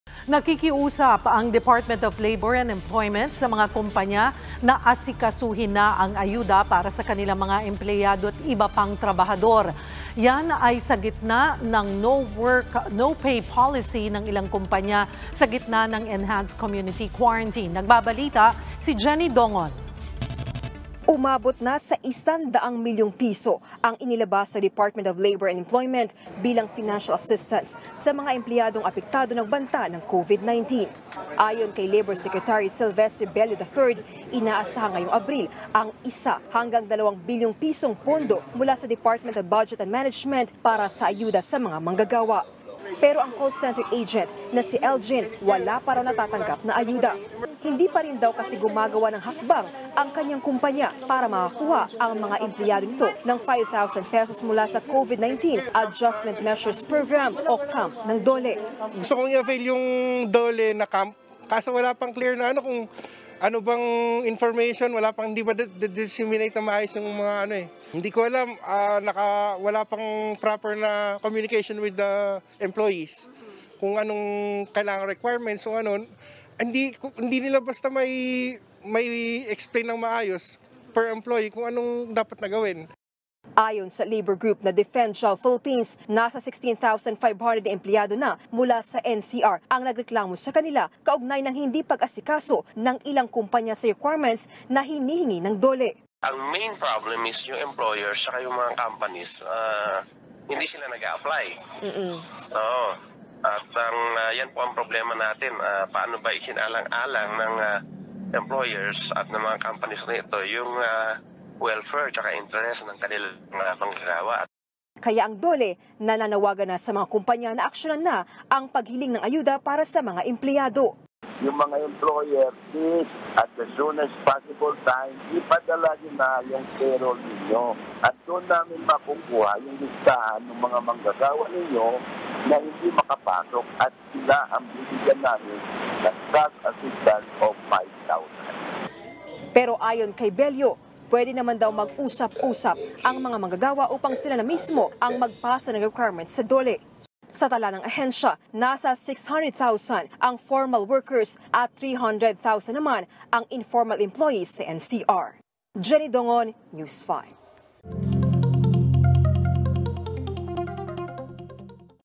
DOLE appeals to companies to ready their assistance for employees
The Department of Labor and Employment appeals to companies to ready their assistance for their employees, amid the “no work, no pay” policy imposed by some during the enhanced community quarantine.